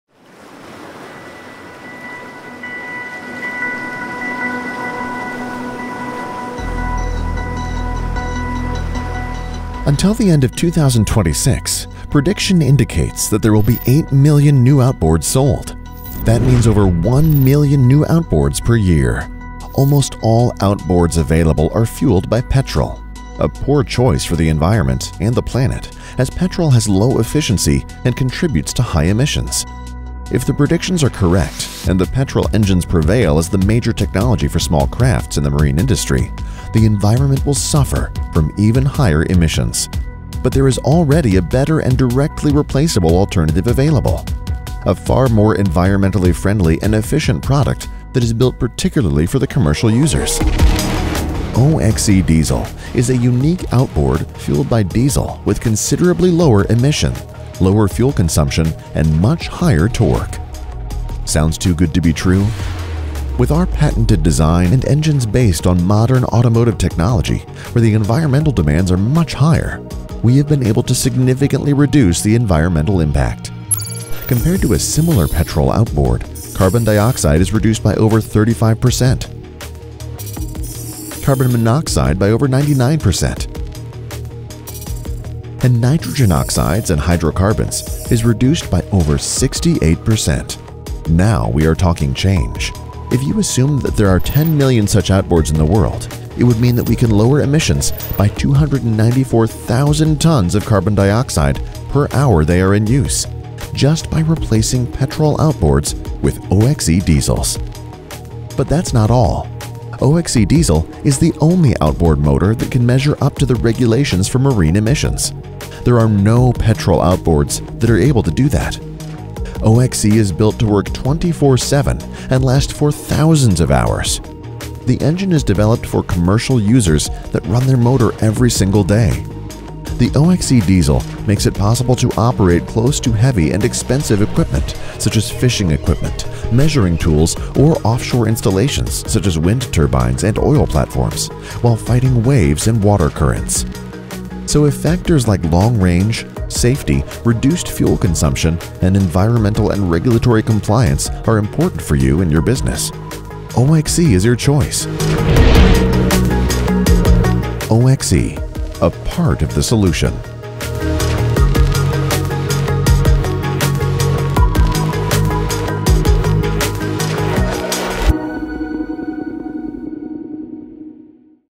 OXE Diesel - A PART OF THE SOLUTION
With Cimco Marine´s patented design and automotive based engine, where the environmental demands are much higher, environmental impact are significantly reduced.

Compared to a petrol outboard of the same hp, Carbone dioxide is reduced by over 35%, Carbone monoxide by over 99% and Nitrogen oxides and Hydrocarbons is reduced by over 68%.